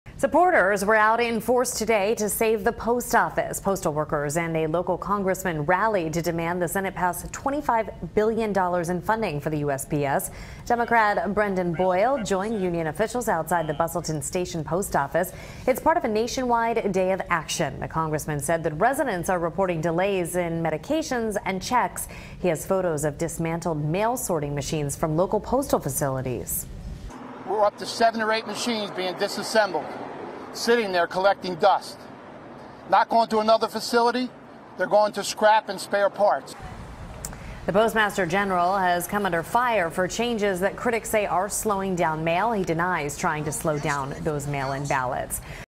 'Save The Post Office' Rally In Bustleton
Supporters called for the Senate to pass $25 billion in funding for USPS.